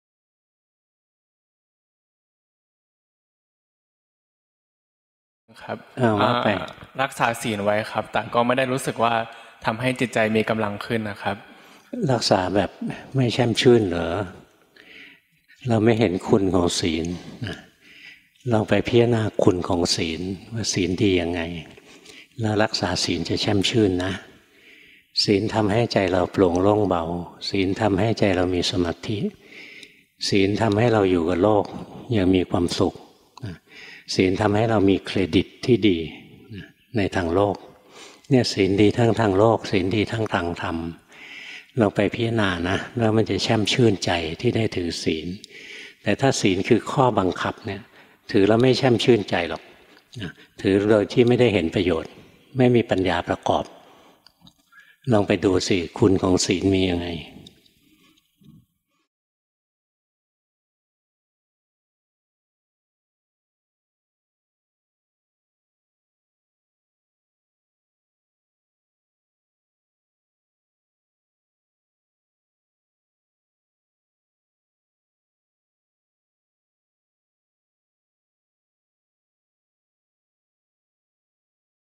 รักษาศีลอย่างไรให้แช่มชื่นใจ :: หลวงพ่อปราโมทย์ ปาโมชฺโช
...ลองไปพิจารณาคุณของศีล ว่าศีลดีอย่างไร
แล้วการรักษาศีลจะแช่มชื่นนะ
ศีลทำให้ใจเราโปร่ง โล่ง เบา
ศีลทำให้ใจเรามีสมาธิ
ศีลทำให้เราอยู่กับโลกอย่างมีความสุข
ศีลทำให้เรามีเครดิตที่ดีในทางโลก
ศีลดีทั้งทางโลก ศีลดีทั้งทางธรรม
ลองไปพิจารณานะ
แล้วมันจะแช่มชื่นใจที่ได้ถือศีล...

บางส่วนของพระธรรมเทศนาหลวงพ่อปราโมทย์ ปาโมชฺโช วัดสวนสันติธรรม บ้านโค้งดารา อ.ศรีราชา จ.ชลบุรี วันที่ ๑๔ มกราคม พ.ศ. ๒๕๖๑

ติดตามข่าวสาร อ่านพระธรรมคำสอน และดาวน์โหลดไฟล์เสียง­ (mp3) ­พระธรรมเทศนาหลวงพ่อปราโมทย์ และฆราวาสผู้ช่วยสอนได้ที่เว็บไซต์ทางการ